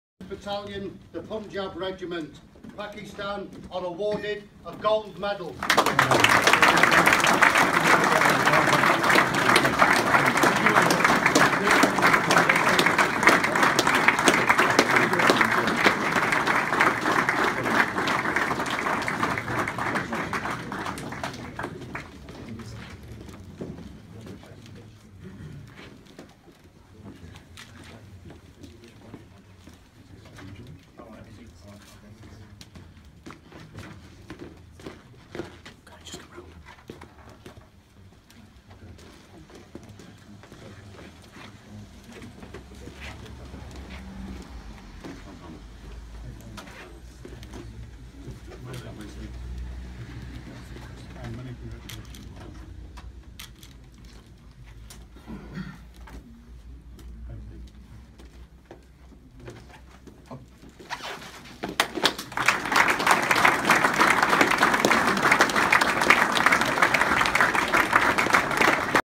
Pakistan Army team won Gold Medal in “Exercise Cambrian Patrol” held in UK.
A total of 131 teams from across the world armies participated in the event known as one of the toughest patrolling test for military personnel. Cambrian Patrol is physically challenging and arduous exercise which concentrates on teamwork for achieving the mission held in the most unforgiving mountainous terrain of mid-Wales, UK, amid tactical and technical challenges, testing leadership, self-discipline, courage and determination of the participants.
This is the fourth time Pakistan Army has won Gold in the Cambrian Patrol winning the competition last year as well.